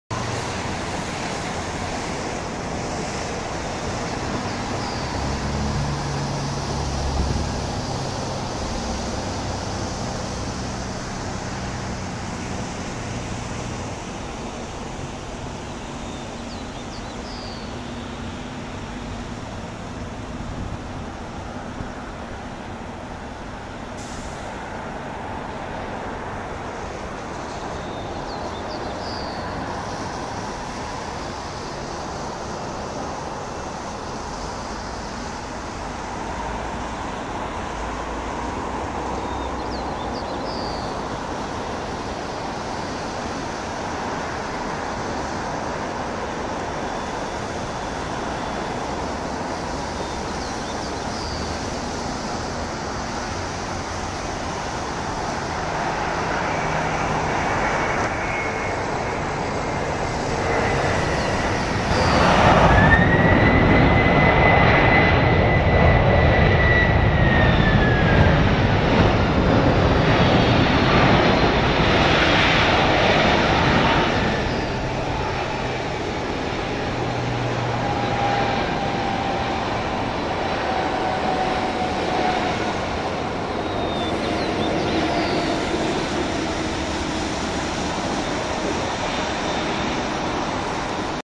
Delta Airlines Boeing 767-300ER [N153DL] landing in PDX
Flight info
Airline: Delta Air Lines
Aircraft: Boeing 767-3P6ER
Reg: N153DL
Livery: Normal
Airport: Portland International Airport
Runway: 28R
Flight #: DL642
Route: NRT-PDX
Date: 4/27/14
Time: 9:01AM
Comments: Delta heavy!